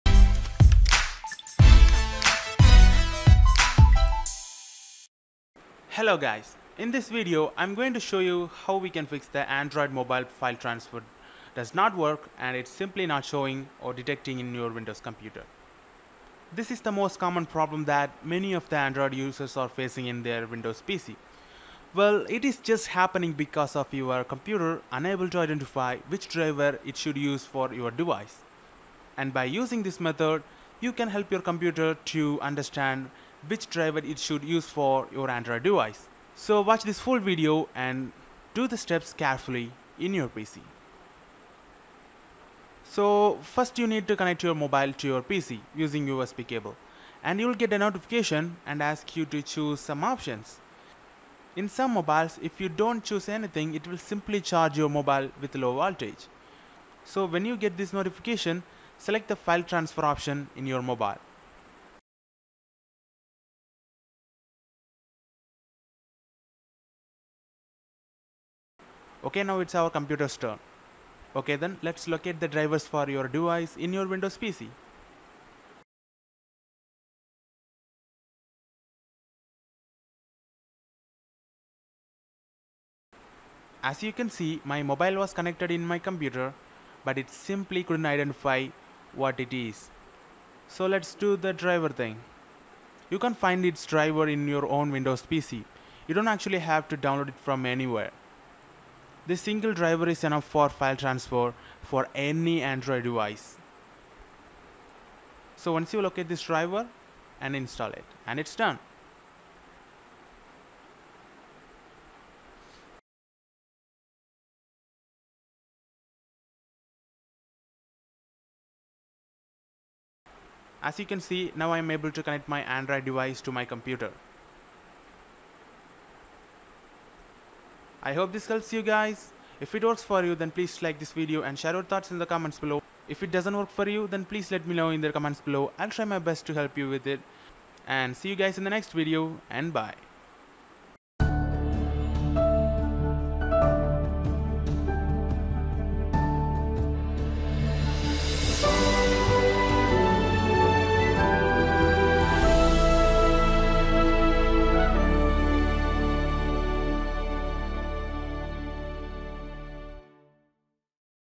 [Solved] Android Device not detected when connected to PC | USB File Transfer
This Video is about How to Fix Android USB File Transfer Device not detected when connected to PC. Most of us want to Transfer files between your computer & Android device right? It's easy to use and manage our files.  But What If your device is not showing when you connect your android mobile using USB Cable? Well, the Solution is Simple. It will mostly happen sometimes when our Computer Coudn't Identify the Device and Which Driver It should Use for it. So when we manually do that for your pc. We can Fix this Problem So easily. Watch this Full Video to Know More about it.

You don't actually have to Download any driver. It will be available in Your PC as a default. Unless If the Manufacturer Provides you with the specific Driver for Your Device.